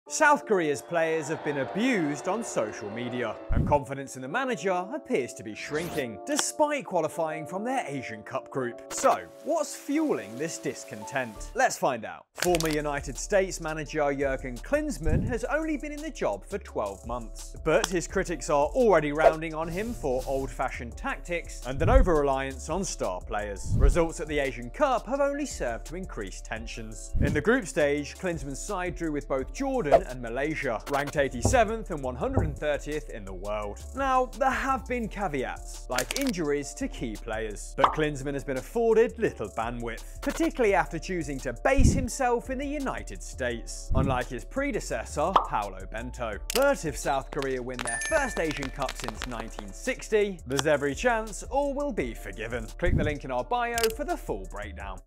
Why South Korea Fans Don’t Like Their Manager 🇰🇷
Welcome to The Athletic FC's YouTube channel, where best-in-class journalism meets video.

South Korea have some of the biggest talents in Asian football, but patience with their national team manager Jurgen Klinsmann is wearing thin. So what's going on?

Additional footage sourced from AP Newsroom